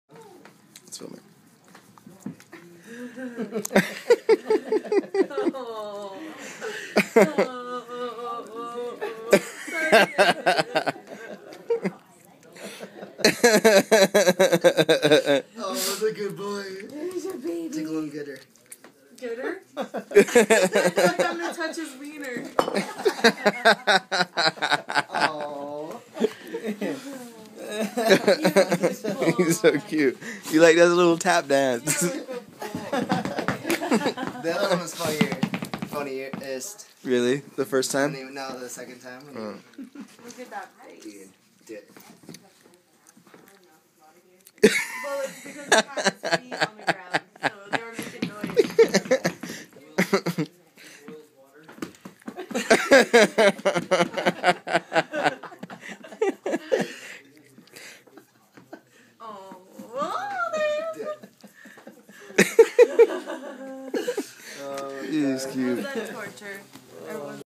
Sundance Being Tickled
Sundance the Doggie can't help but tap dance his feet from being given some tickle loves.